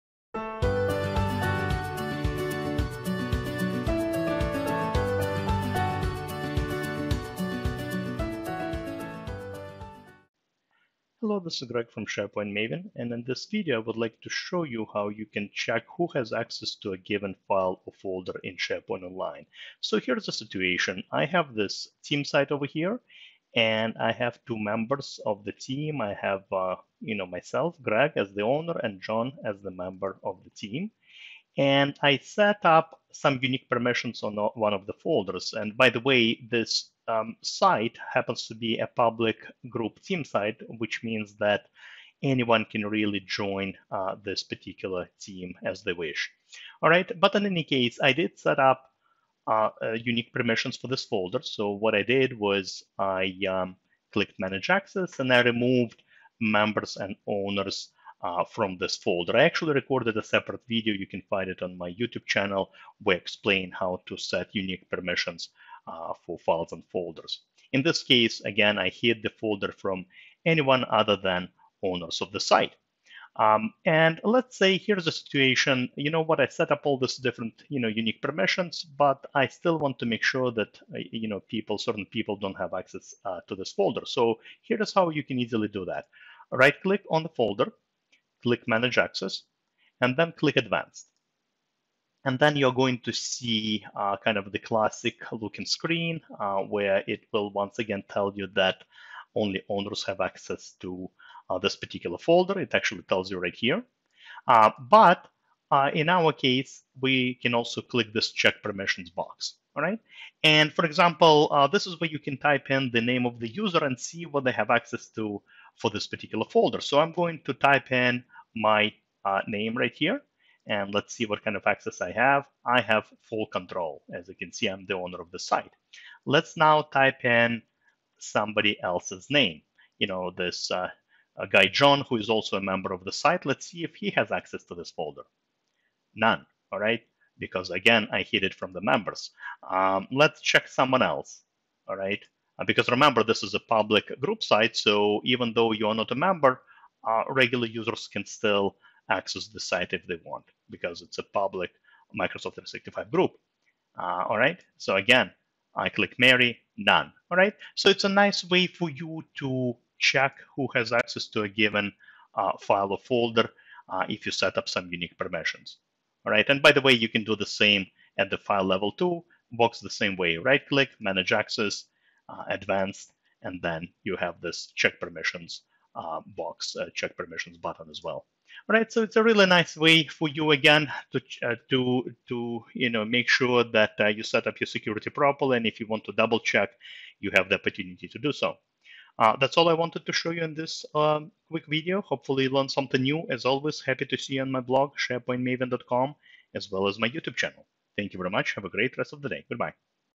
How to check permissions for a SharePoint file or folder via the Check Permissions feature
Did you know you can easily check who has access to a given file or folder? In this video, I explain how to check permissions using the "Check Permissions" feature. Here is a post on the same topic as well